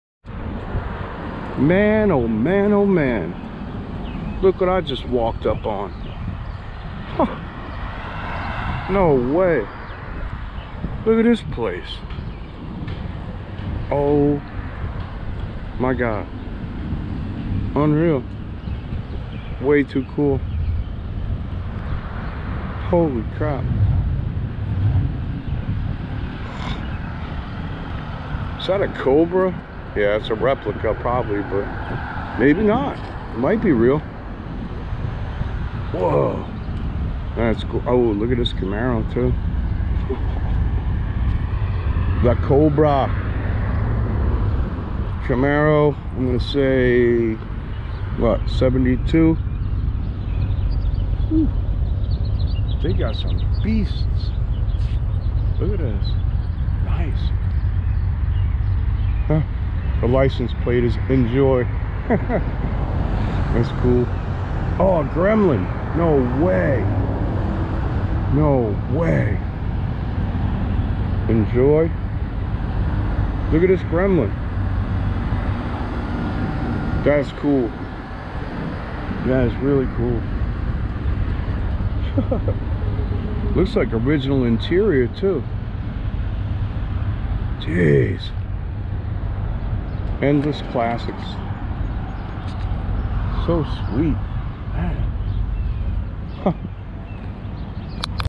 Endless Classic Cars - Discovered a Cool Place!
While out running errands and making large withdrawals from banks where I hold no accounts, I stumbled upon this auto dealer with some rare and interesting gems.